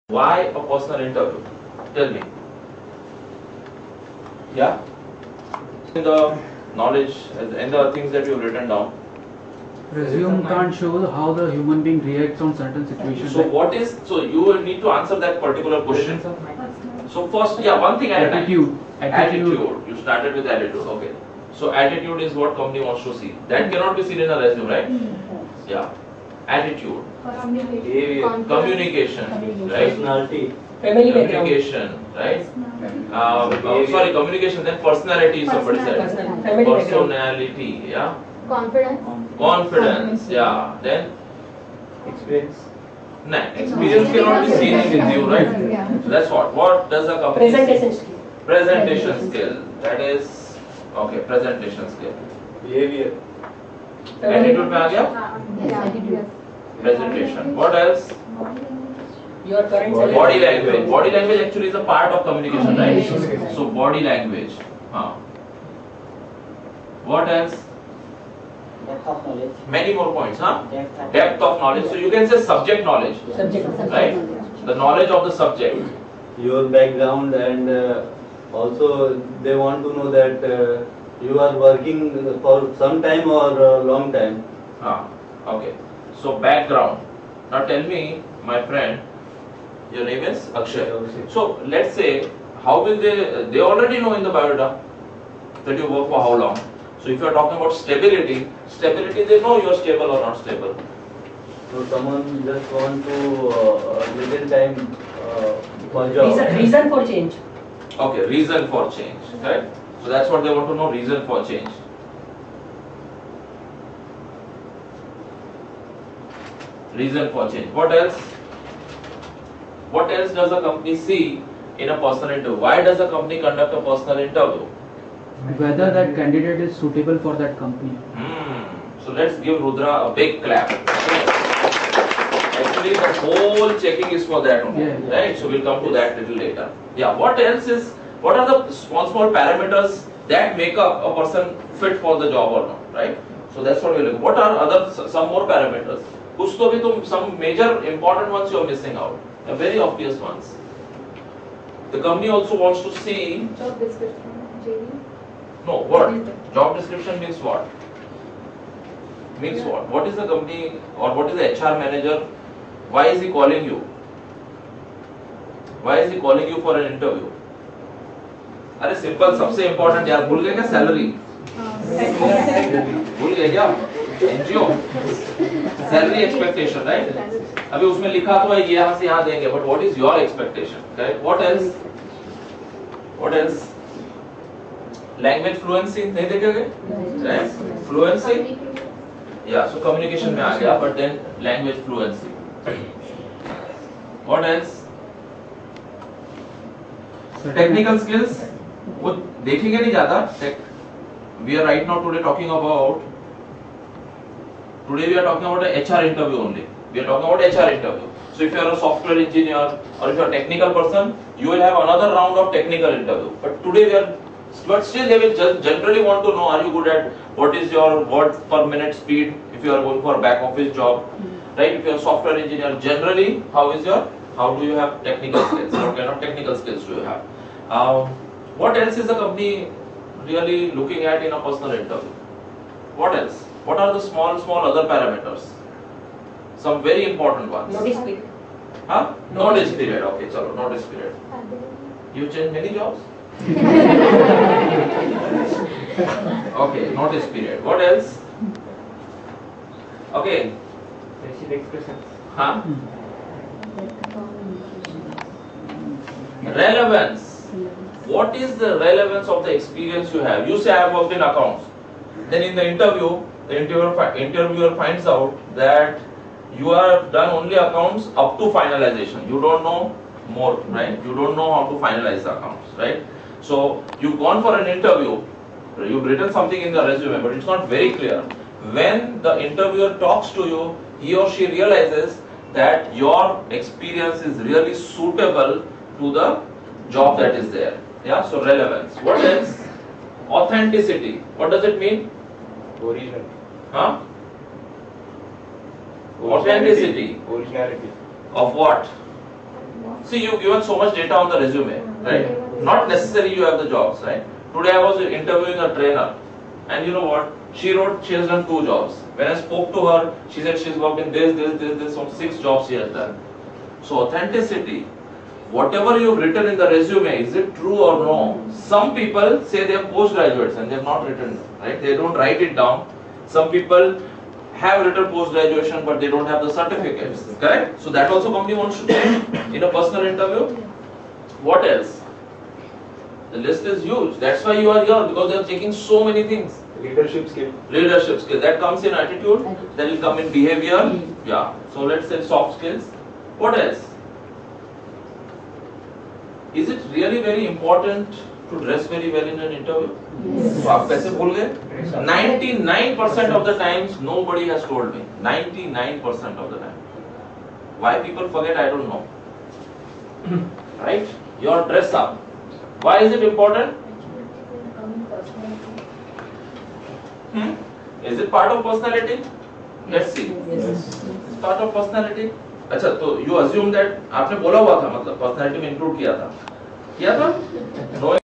Interview Tips (Why does HR conduct a Personal interview?)
What parameters is the HR/manager looking at when an interview is being conducted? What is the objective of a personal interview? BM English Speaking Interview Training.

A Step by Step Guide To Clear Any Interview With Confidence,

Why a personal interview?

A: in the knowledge of the things that you have written down
  resume cant shows how the human being react on certain situations
  so what is you will need to answer the particular situations.

Suggestions:

1.ATTITUDE
attitude is what company wants to see that cannot be seen on a resume.

2.Communication
body language actually is a part of communication right

3.Background
question where you get to summarise your background in order to point out the most relevant skills, expertise and accomplishments that make you the best hire.

4.leadership skills
leadership skills that come in attitude that will  come in behaviour

5.Personality
dress very well for an interview best soft skills knowledge.